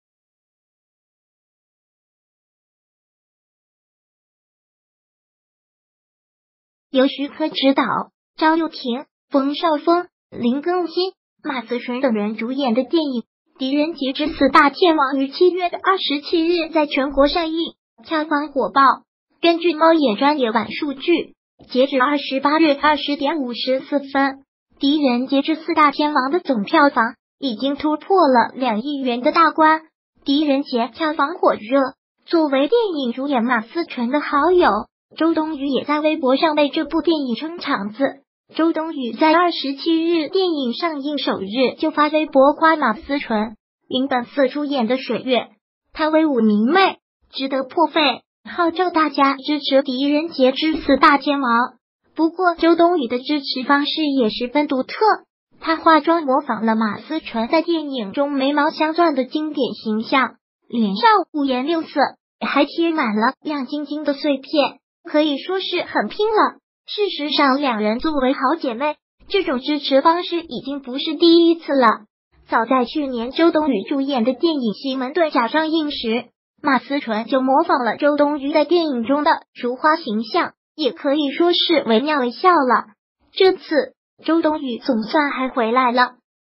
狄仁杰票房破2亿，周冬雨模仿马思纯片中形象，脸上镶钻了
由徐克执导，赵又廷、冯绍峰、林更新、马思纯等人主演的电影《狄仁杰之四大天王》于7月27日在全国上映，票房火爆。 根据猫眼专业版数据，截止28日20点54分，《狄仁杰之四大天王》的总票房已经突破了2亿元的大关。 “狄仁杰”票房火热，作为电影主演马思纯的好友，周冬雨也在微博上为这部电影“撑场子”。周冬雨在27日电影上映首日就发微博夸马思纯，“零本色出演的水月，她威武明媚，值得破费”，号召大家支持《狄仁杰之四大天王》。 不过周冬雨的支持方式也十分独特，她化妆模仿了马思纯在电影中“眉毛镶钻”的经典形象，脸上五颜六色，还贴满了亮晶晶的碎片，可以说是很拼了。 事实上两人作为好姐妹，这种支持方式已经不是第一次了。早在去年周冬雨主演的电影《奇门遁甲》上映时，马思纯就模仿了周冬雨在电影中的“如花”形象，也可以说是惟妙惟肖了。这次，周冬雨总算还回来了。